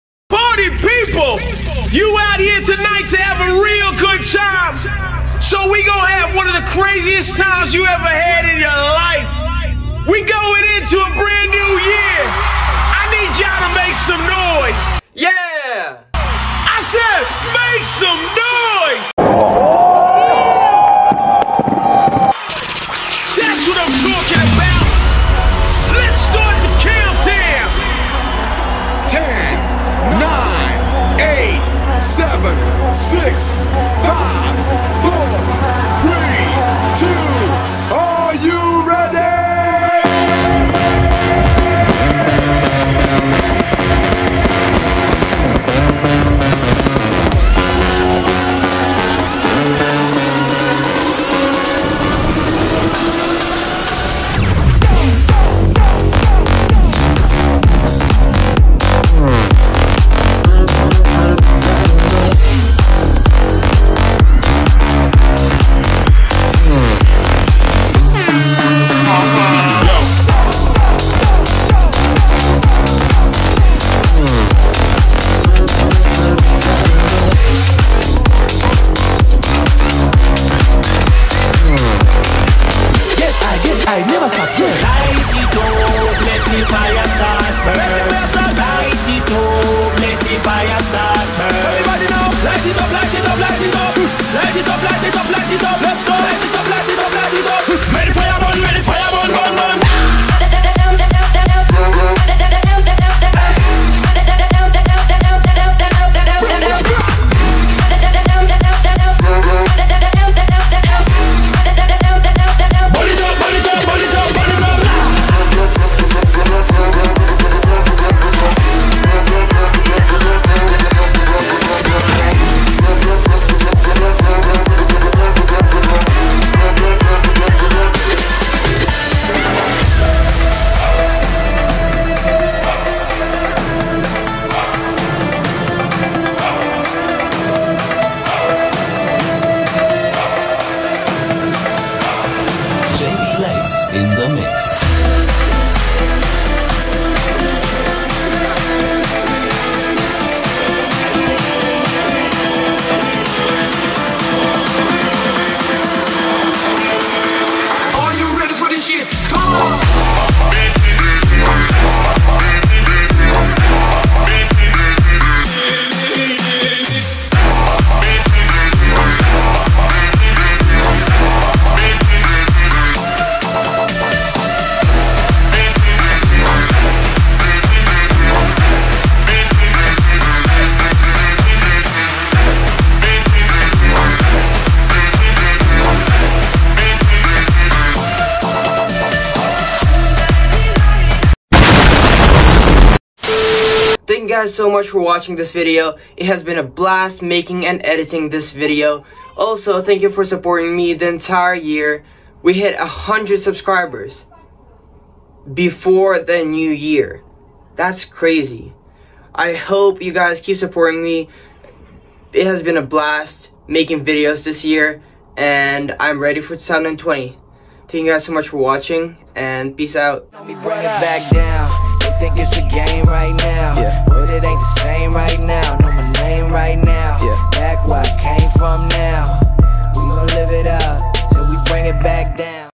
New Year Celebration in Russia 2020! Flips, Skiing, and Sledding
Thank you for supporting me this year! I have had a lot of fun making videos for you guys! Let's break 400 by the end of this year!